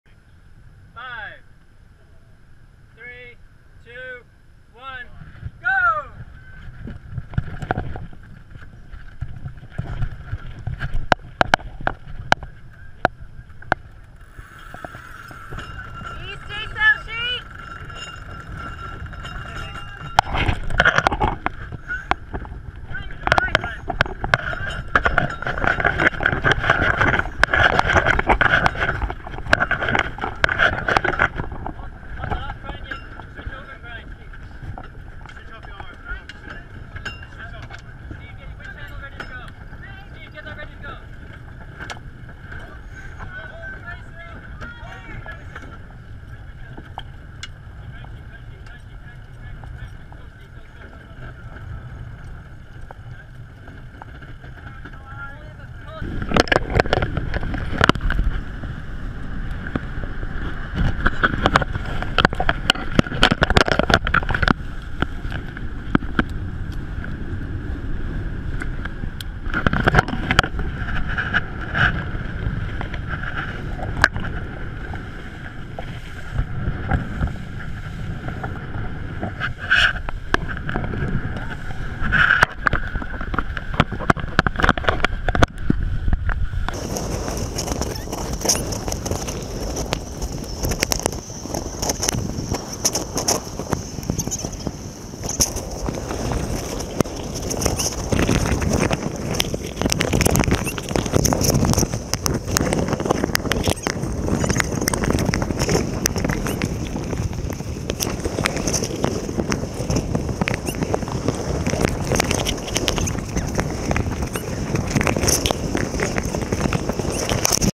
Henri Lloyd starts Race 8, Brisbane, Australia to Singapore
Henri Lloyd, an entry in the Clipper Round the World Yacht Race, starts the race from Brisbane, Australia to Singapore.